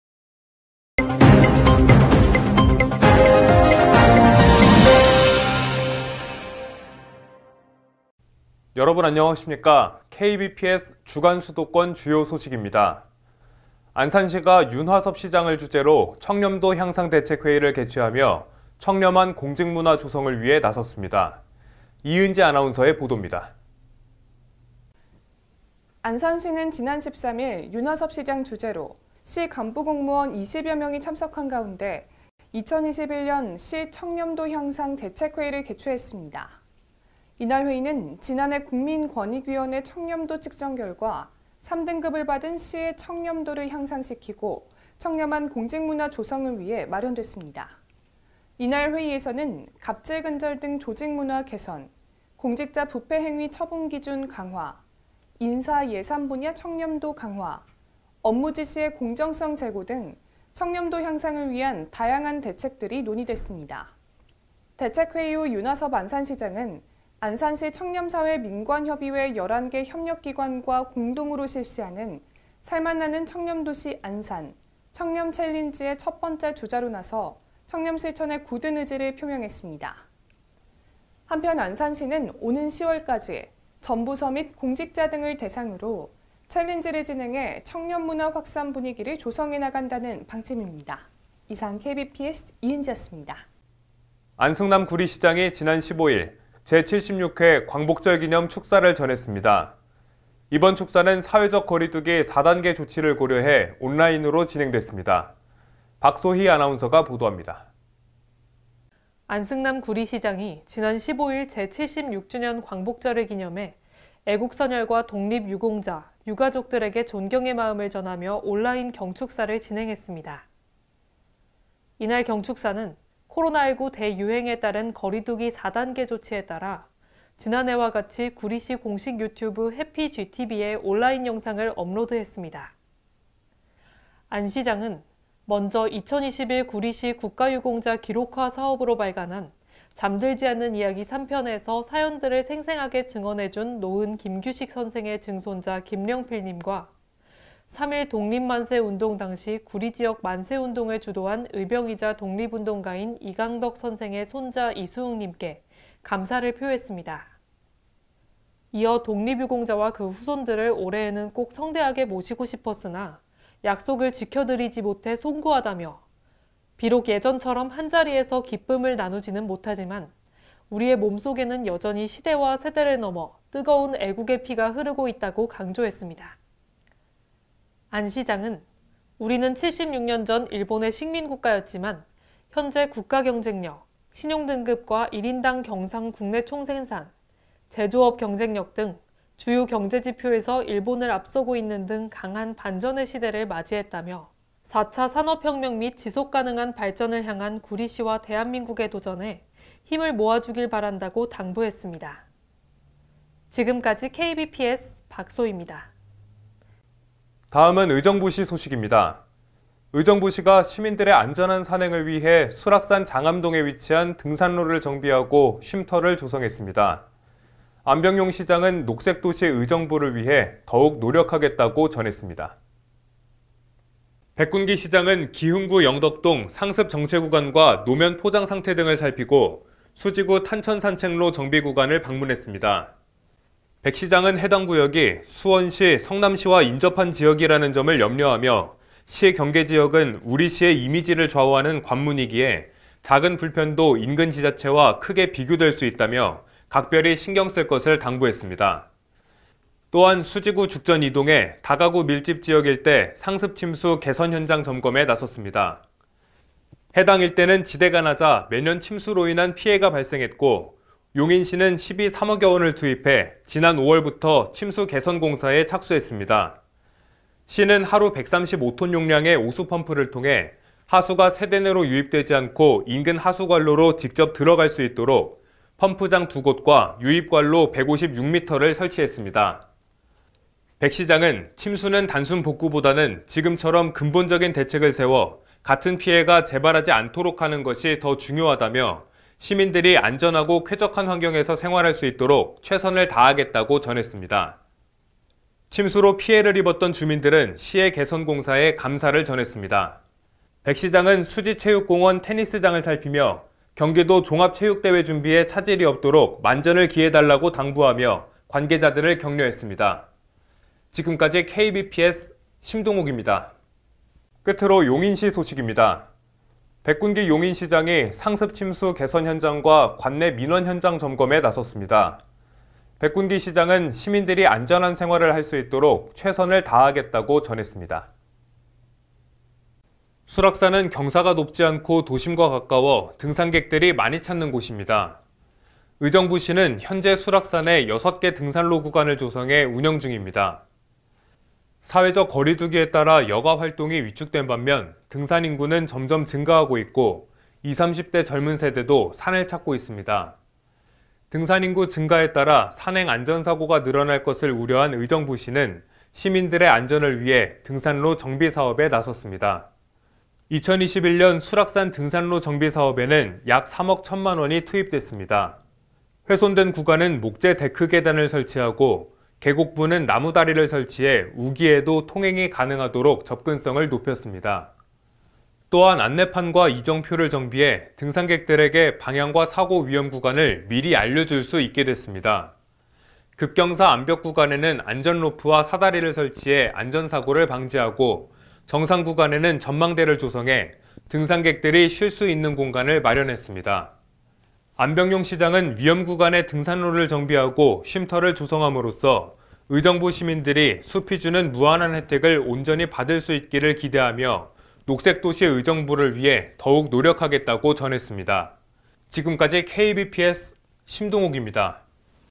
[2021년 8월 3주] KBPS 주간 수도권 지역별 주요 소식
- 안산시, 윤화섭 시장 주재 청렴도 대책회의 개최
- 안승남 구리시장, 제 76회 광복절 기념 축사 전달
- 의정부시, 수락산 일대 등산로 정비 사업 완료
- 백군기 용인시장, 관내 상습 침수 개선 현장·민원 현장 점검